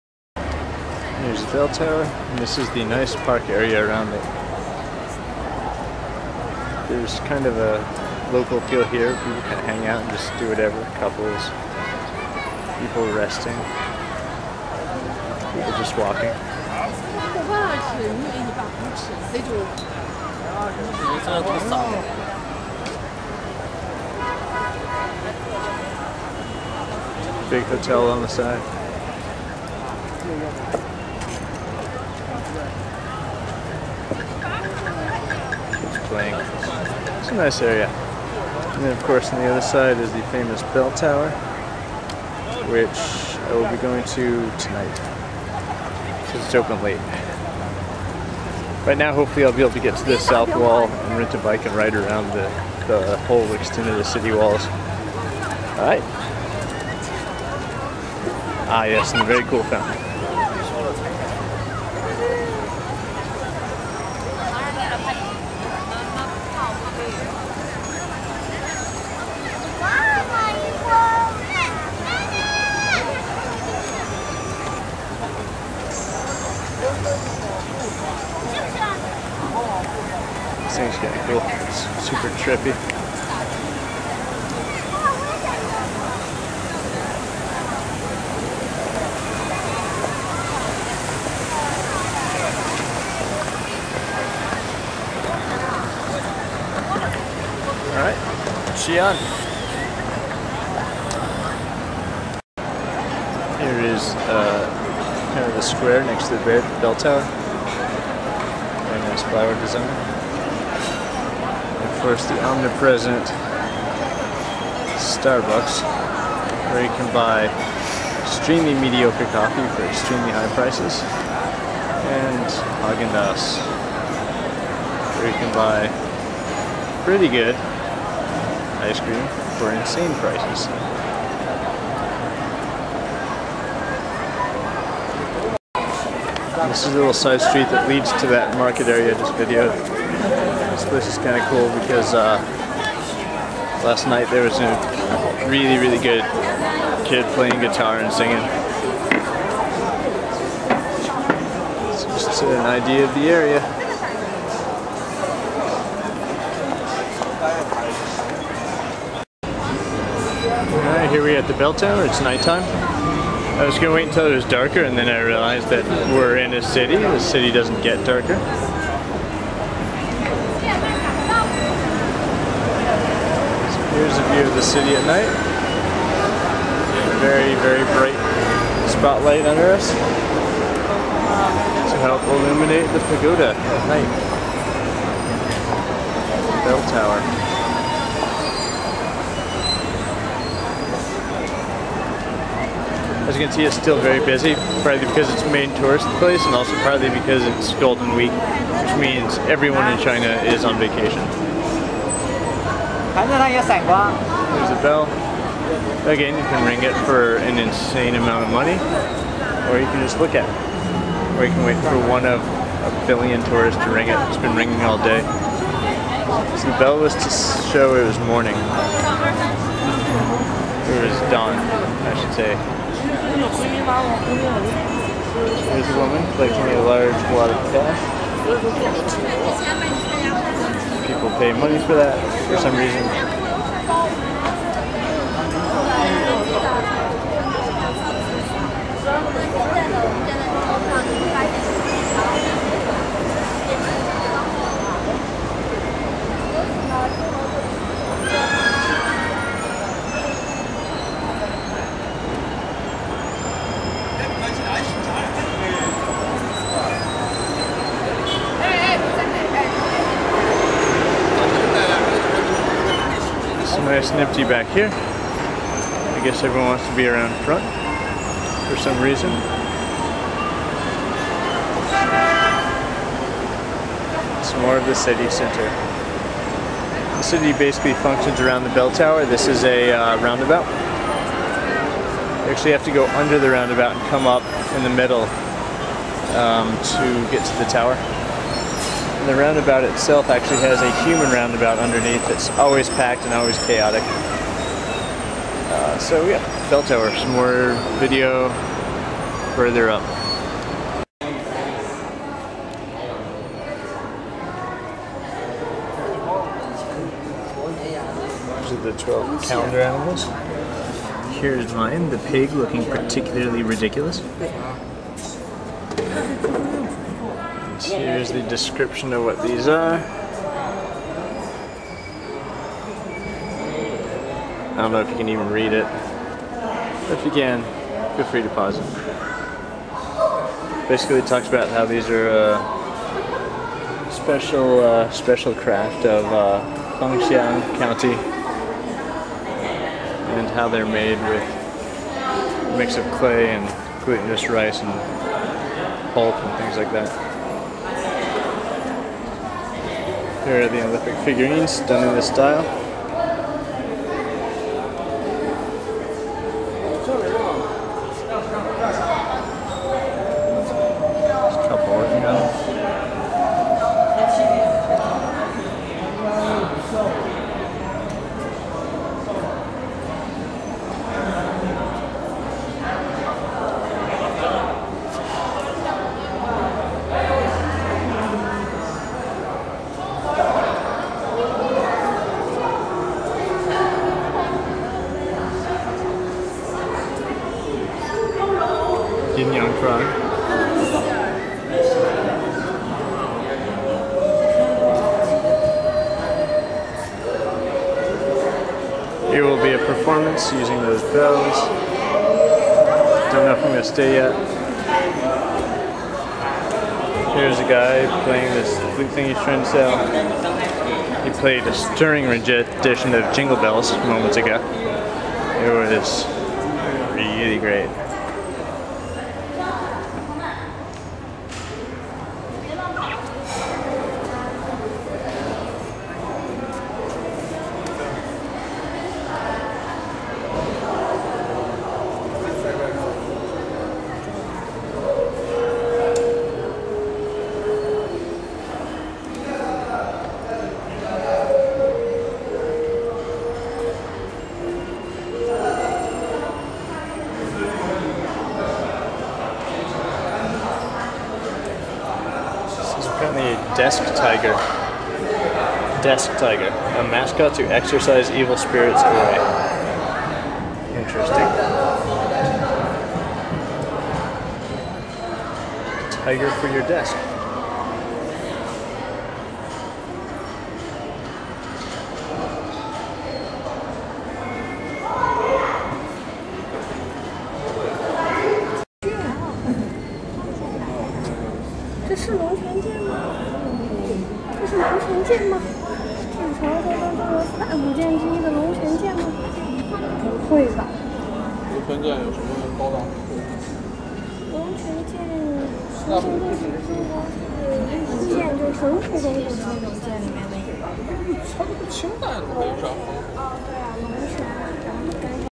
Xian - Bell Tower 1
This is the video 1 of 3 of the Drum Tower's twin: The Bell Tower.  A significant part of these videos is the performance in the tower.  The drum performance was pretty bad, in my opinion, which is why there's only a short part of that performance on the Drum Tower video.  This one is much better, so I got most of it on video, which takes up a good amount of time.